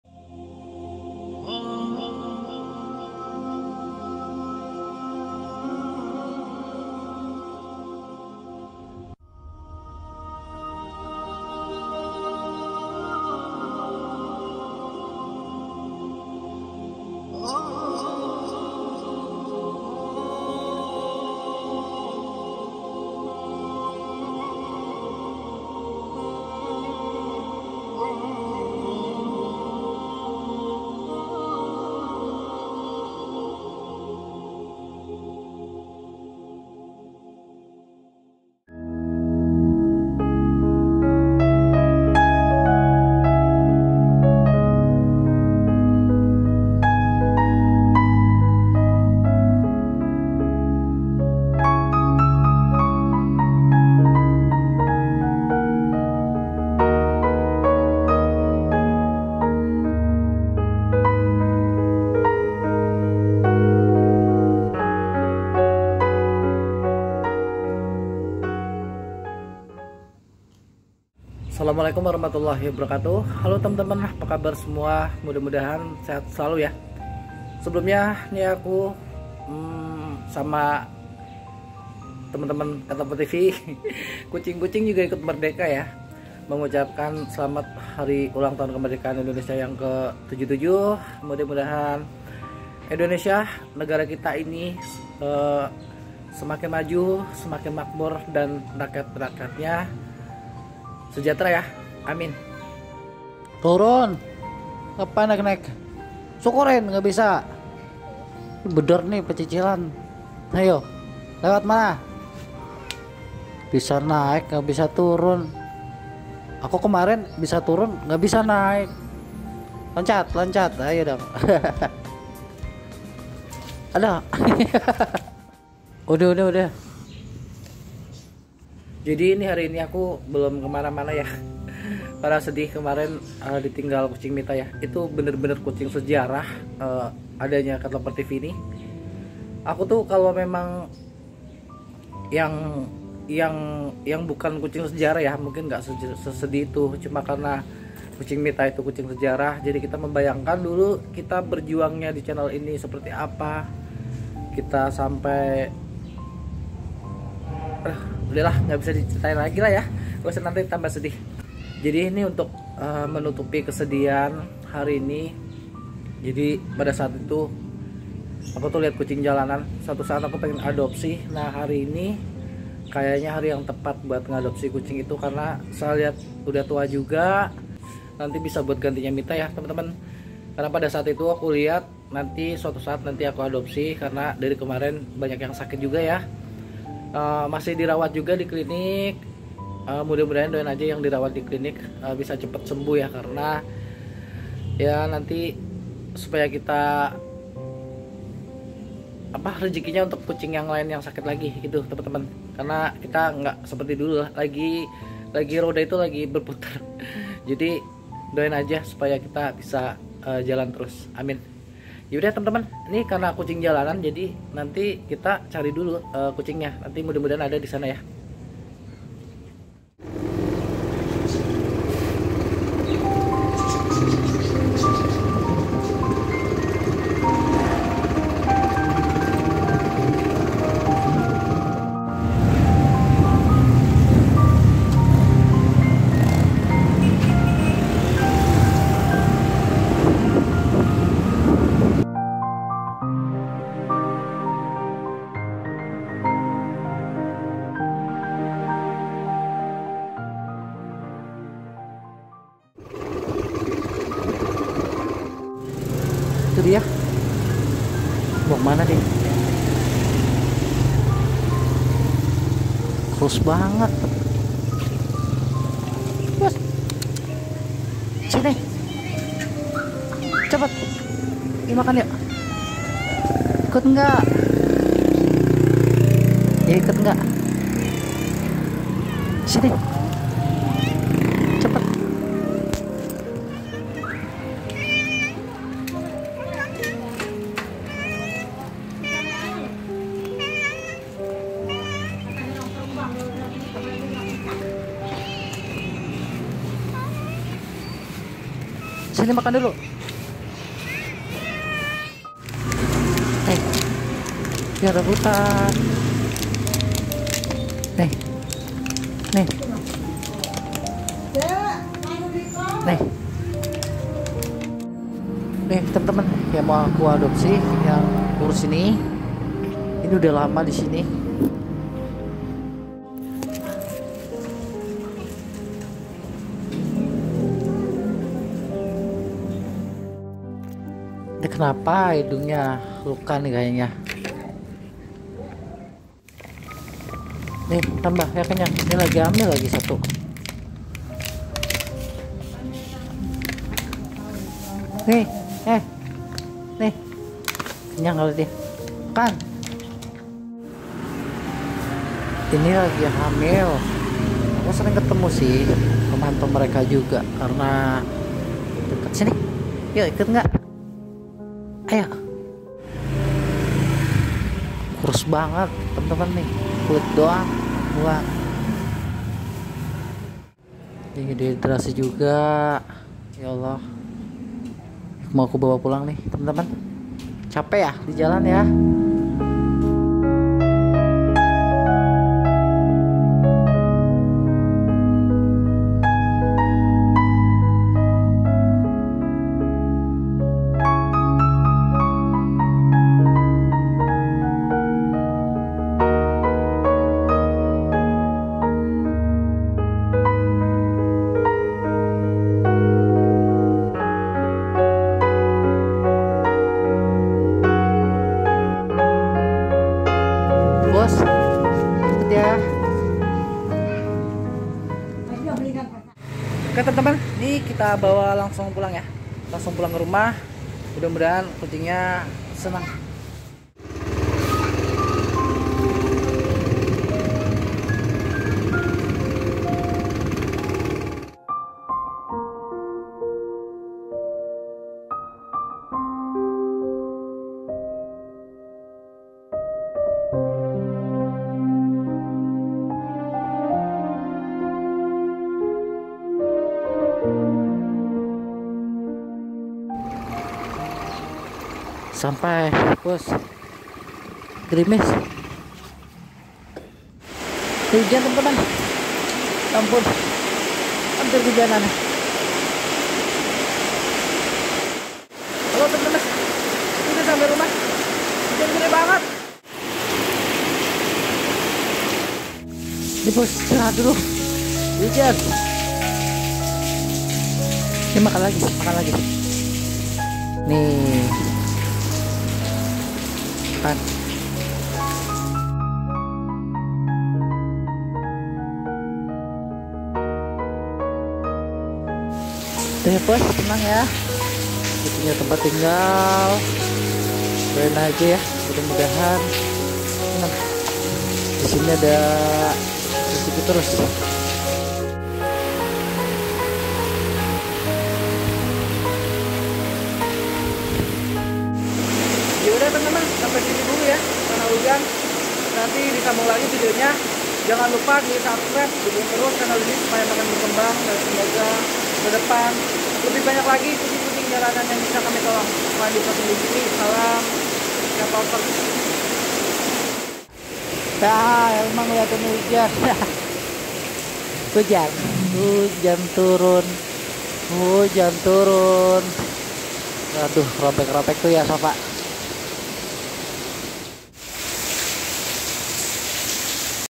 Masya Allah Kucing Tua Kurus Ngejar-Ngejar Mau Di Adopsi Mengharukan Saat Di Bawa Pulang..!
Kucing Kurus ini adalah kucung jalanan yang ada di sekitaran bintara jaya bekasi barat.
Sebenarnya Sudsh lama kami ingin mengapdosinya tetapi karena kemarin banyak yang sakit sehingga kami baru bisa mengadopsinya hari ini.

Kucing ini setiap hari meminta jeroan ikan di tukang sayur untuk menyambung hidupnya kadang juga datang ke warteg untuk meminta makanan sisa tetapi ,

karena sangking banyaknya kucing sehingga terkadang dia tidak kebagian dan badannya sampai kurus karena kurang makan.

Mulai hari ini Insyaallah kami akan merawatnya dan menyediakan makan setiap hari supaya dia bahagia di masa Tuanya.

Tlp/wa.      : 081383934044 khusus penting / Darurat